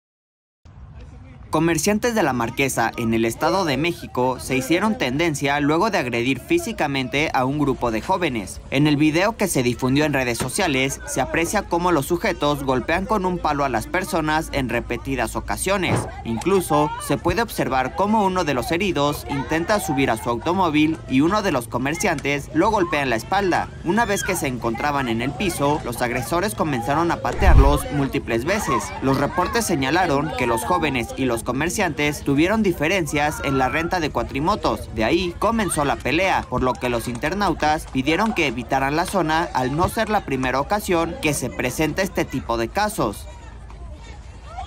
Comerciantes de LaMarquesa golpean y patean a grupo de jóvenes
Comerciantes de LaMarquesa en el EstadodeMéxico agarraron a golpes a un grupo de jóvenes con un palo de madera e incluso los patearon en múltiples ocasiones.

Reportes indicaron que el conflicto se dio luego de que los dueños de unas cuatrimotos no quisieran devolverle su cambio a las otras personas tras rentarlas.

Uno de los afectados terminó en el hospital al presentar contusiones, sin embargo, no hubo ningún detenido. 😰